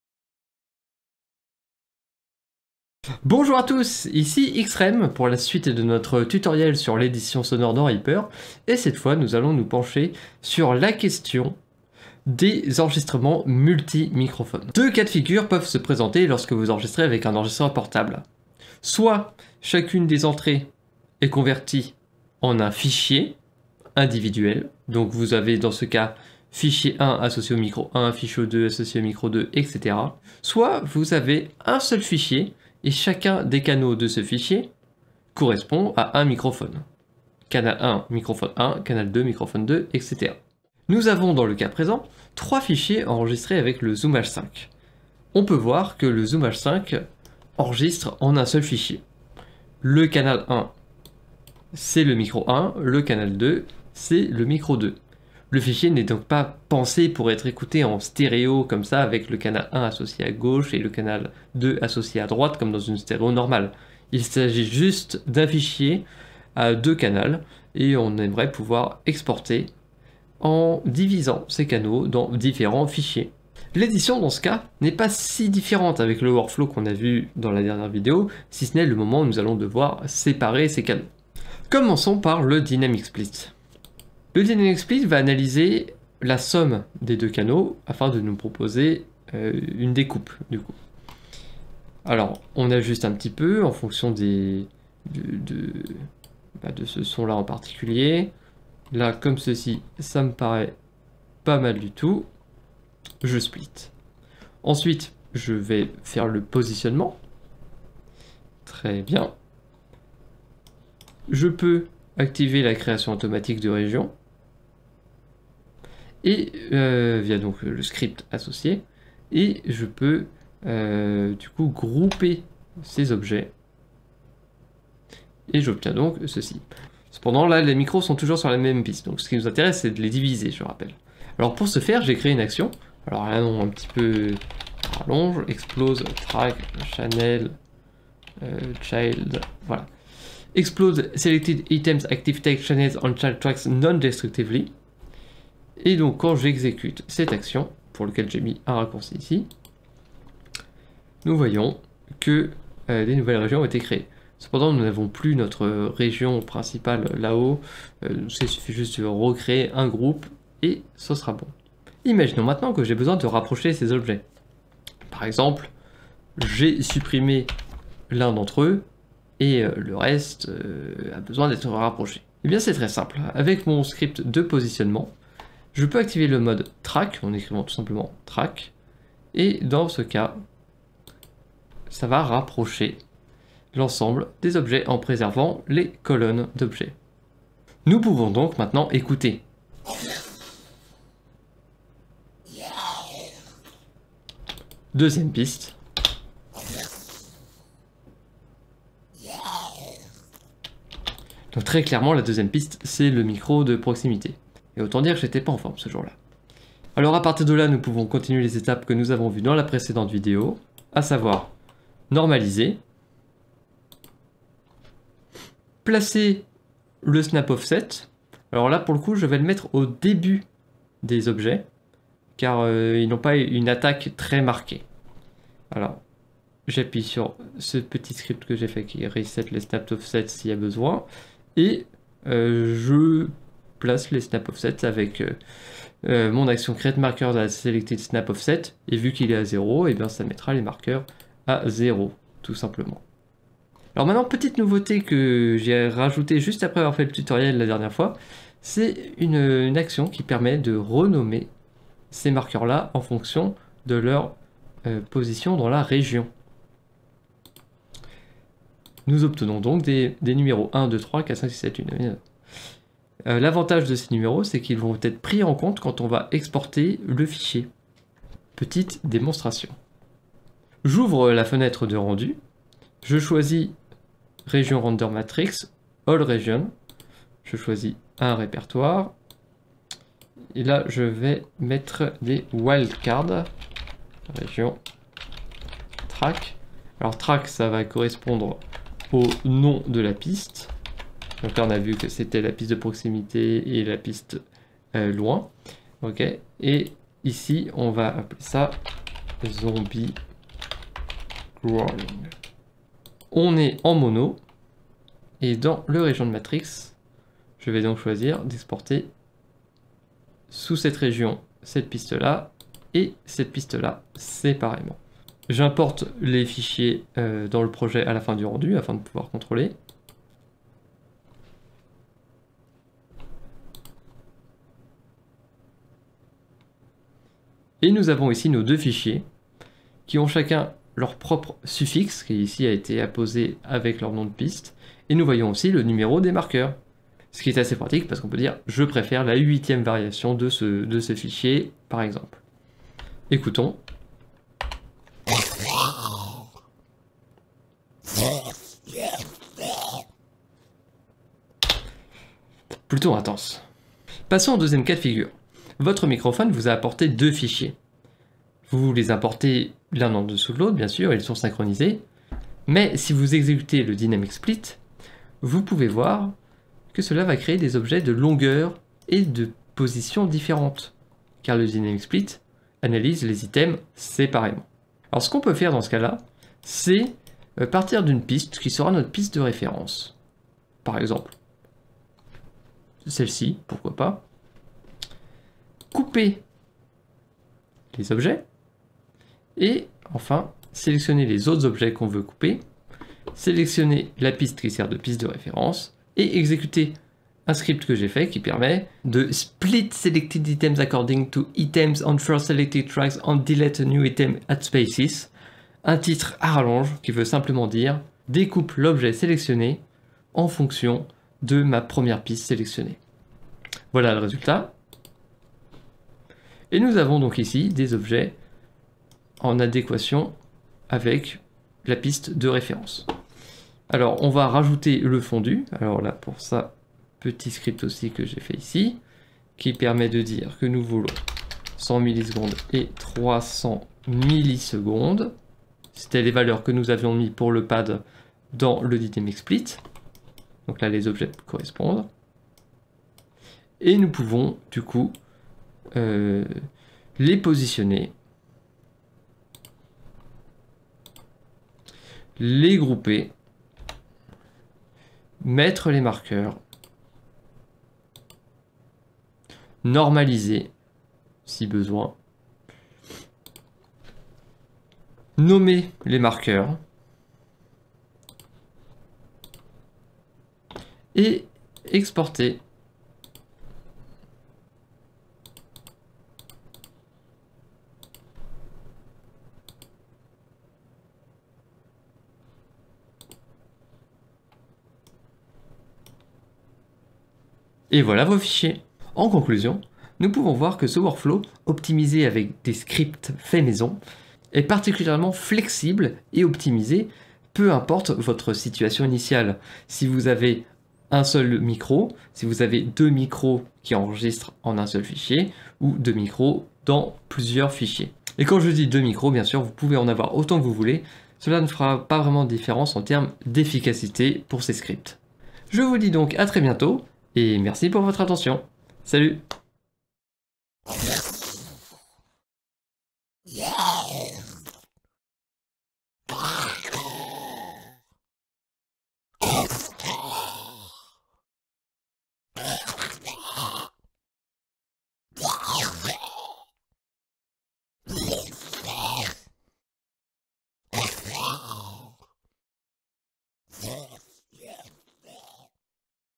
Sound Effects / Sample Library Editing avec des scripts REAPER (version multi-pistes)
This is a follow-up of my previous video, to show how to edit on multi-mics recording.

Quick demo of custom scripts I made for my sound effects editing workflow in REAPER.
Sorry for lack of dynamism, I was experimenting a new tutorial workflow, I had to record this many times before it works flawlessly.

(Note: there is few tiny errors in the video which I couldn't fix cause, like the fact I forgot to add "multi-tracks" version on the title, but nothing too dramatic).

01:21 - One File - Editing
05:54 - Multiple Files - Editing
08:09 - Conclusion
08:40 - Outro

 *Scripts shown in the video:*

- X-Raym_Create regions from items groups (background).lua
- X-Raym_Normalize selected items by groups with true peaks.lua
- X-Raym_Create markers at selected items snap offset.lua
- X-Raym_Set selected items position and order.lua
- X-Raym_Set selected items snap offset to max peak position.lua

- X-Raym_Rename markers in time selection or at edit cursor position with patterns_v2.lua

- X-Raym_Reset item snap offset.eel
- X-Raym_Split selected items according to items on first selected track and delete new items at spaces.lua

*Associated Scripts pack*

Sounds edited in the video were part of the Creature Field Recording Slack Slack Team Crow Source.

Thanks to sleeping_mary for having shared her computer and DSLR so I can make this tutorial!